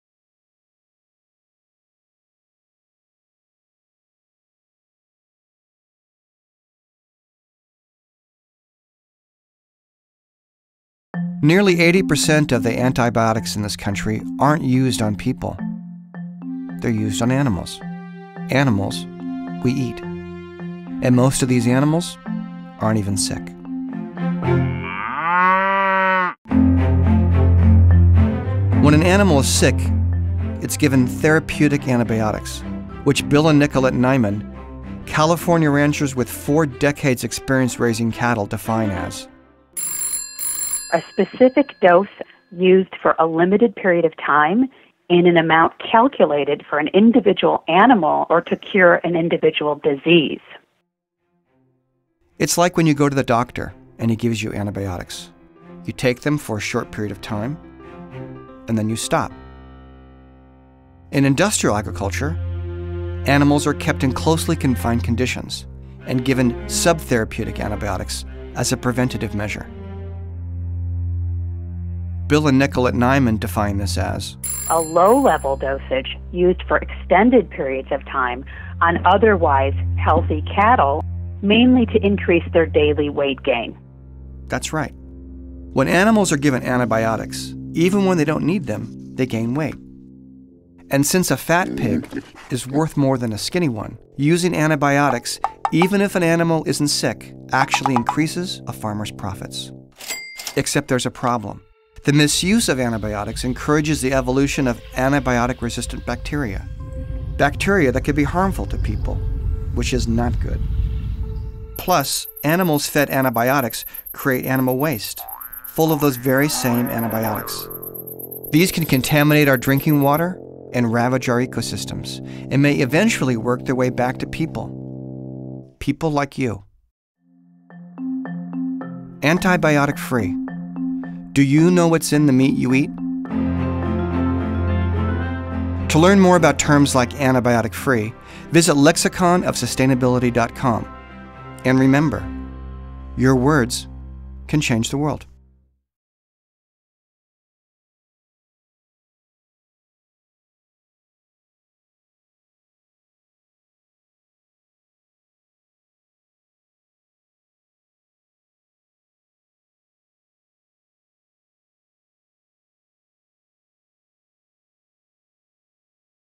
Antibiotic Free
Nearly 80% of the antibiotics in this country aren't used on people. They're used on animals, animals we eat. And most of them aren't even sick. Bill and Nicolette Niman share their insights on the growing movement to remove sub-therapeutic antibiotics from American beef.

A Rumplefarm, LLC Production
Produced by Laura Howard-Gayeton
Written and Directed by Douglas Gayeton
Edited by Pier Giorgio Provenzano

Bill Niman
BN Ranch
Bolinas, CA
Bill Niman is a cattle rancher in Northern California, proprietor of BN Ranch, and Founder of the natural meat company Niman Ranch, Inc. He was a member of the Pew Foundation's National Commission on Industrial Farm Animal Production, which released recommendations for reform of the nation's livestock industry in April 2008.
Nicolette Niman
BN Ranch
Bolinas, CA
Nicolette Hahn Niman is an attorney, author and livestock rancher. She is a passionate advocate of sustainable food production.
Antibiotic free -- "FRESH AIR + SUNSHINE + EXERCISE = HEALTHY ANIMALS (NO DRUGS REQUIRED)"
- Bill and Nicolette Niman, BN Ranch in Bolinas, CA

Sub-therapeutic antibiotics -- "Low level dosages used for extended periods of time on otherwise healthy cattle, mainly to increase their daily weight gain."

Therapeutic antibiotics -- "A specific dose used for a limited period of time, in an amount calculated for an individual animal to cure a particular disease"-

CAFO -- "Systems where livestock are kept in very crowded situations—usually in some kind of barn or facility—and fed a diet that consists mainly of grain, soybeans, and supplements (for milk cows, this include some forage like hay)."
-LOCAL: The New Face of Food and Farming in America, by Douglas Gayeton